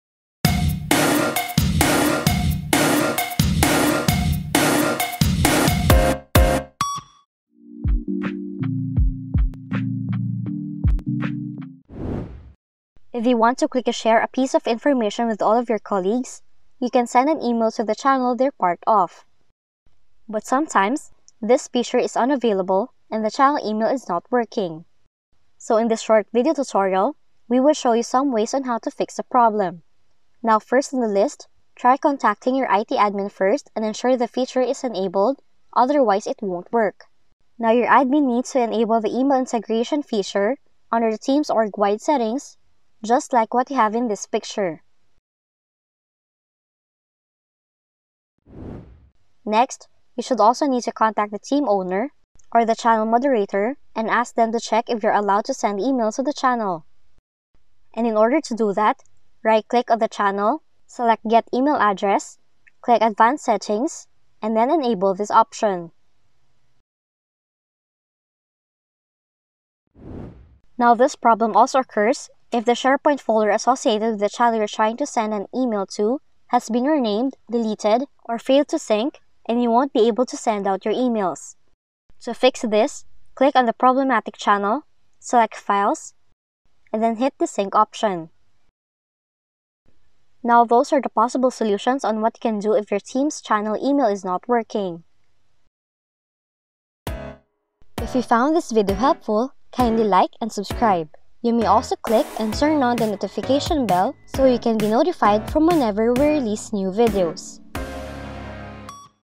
FIX: Microsoft Teams Channel Email Not Working
It can be quite frustrating when you want to quickly share some information with your Team mates, but find that the channel email isn't working in Teams. In this tutorial, we'll show you several reasons it might not be working and how to troubleshoot the issue.

1. Make sure the feature is enabled. (You may have to contact your I.T. department.)

2. Contact the Team owner or channel moderator to check settings on their side.

3. Check to make sure the SharePoint folder name hasn't been renamed, moved, or deleted.